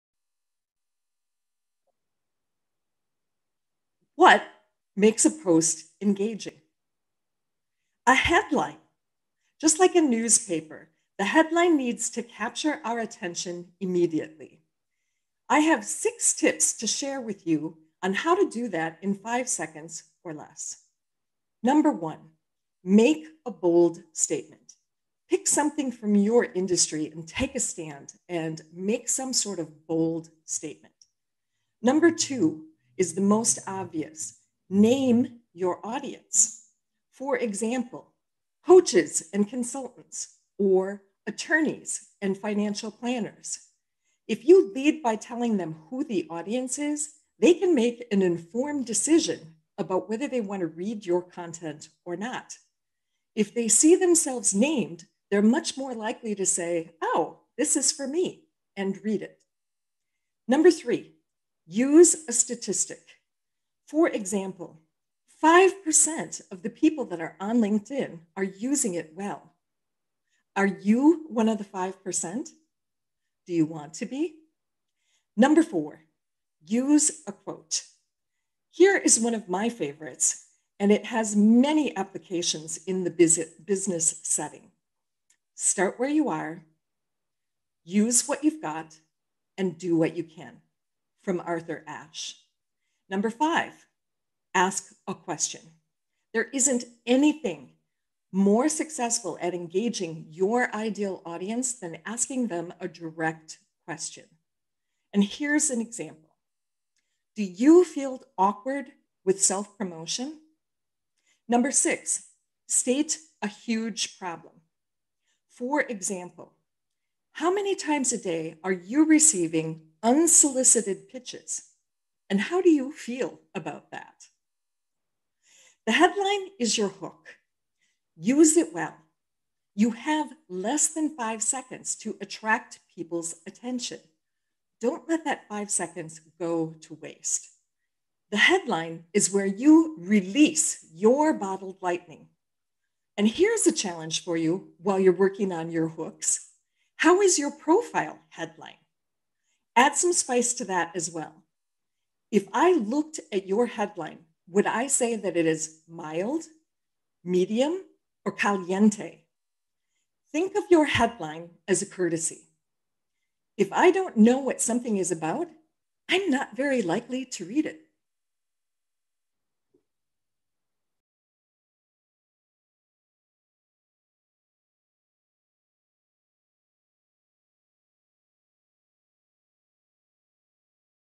6 Tips for Making a Post Engaging Immediately
What makes a post engaging?
A headline
Just like a newspaper, the headline needs to capture attention immediately
3 - 5 seconds is all you get
The headline is where you release your bottled lightning
Listen in for all 6 Tips